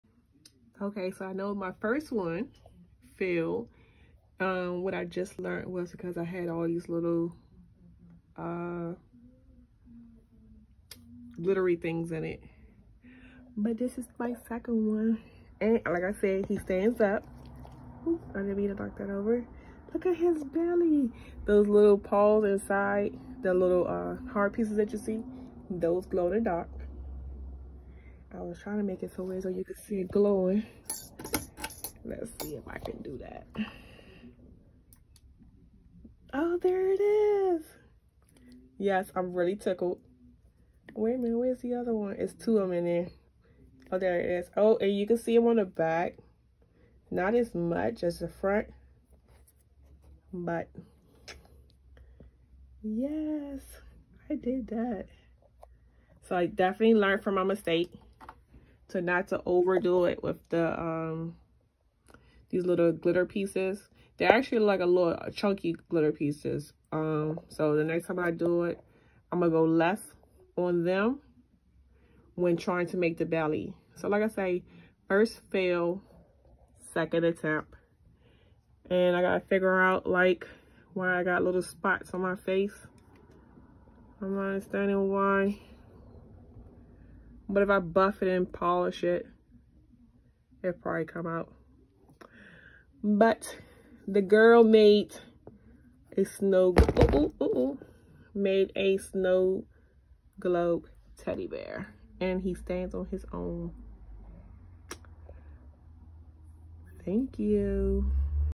Snow globe￼ teddy bear
My fail and my win.
Had to redeem myself from my first fail, I had put to much chunky glitter in the resin￼